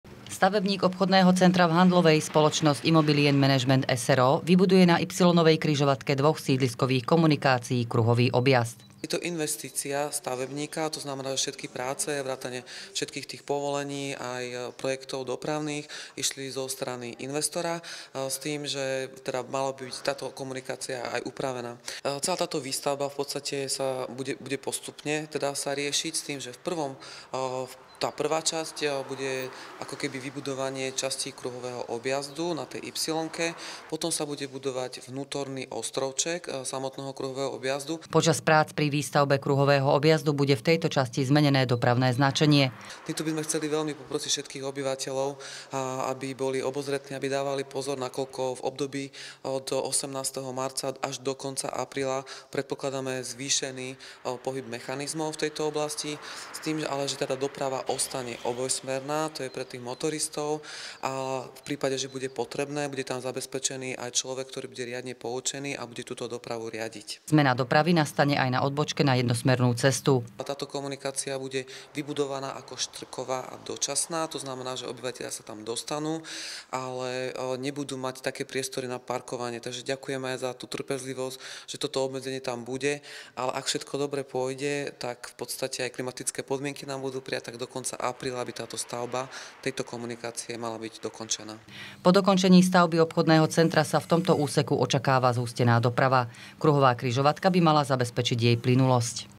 Vybudujú kruhovú križovatku
Investor stavby budúceho obchodného centra v Handlovej začal v týchto dňoch s prípravami na  vybudovanie kruhového objazdu na križovatke Okružnej ulice a Morovnianskej cesty.